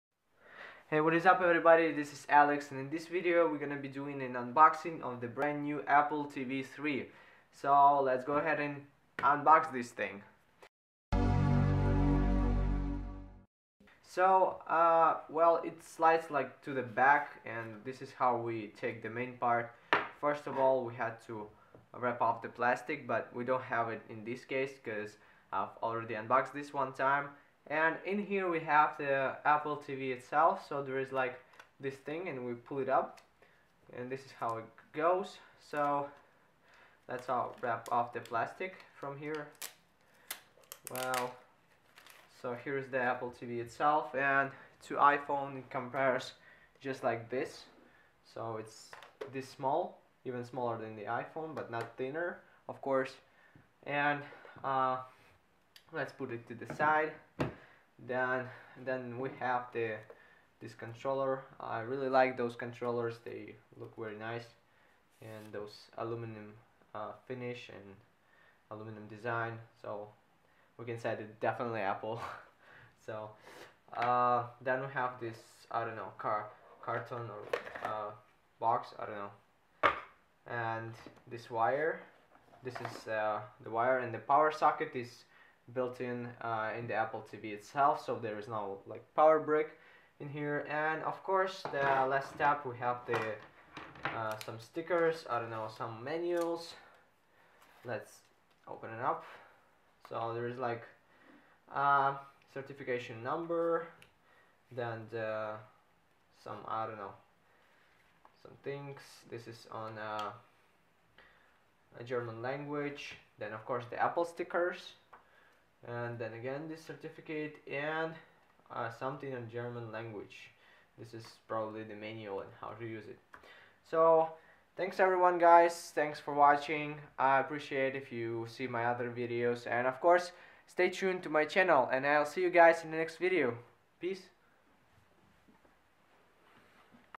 Apple TV 3 Unboxing new review 3 3rd generation 3G 2012 1080p model
In this video I do an unboxing of the Apple TV 3 3rd Generation 3G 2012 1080p model. This newest Apple TV features the Apple A5 processor which allows it to output 1080p video and is very similar vs Apple TV 2 with the same overall design, HDMI, Optical Audio, a MicroUSB port, Ethernet and Wi-Fi.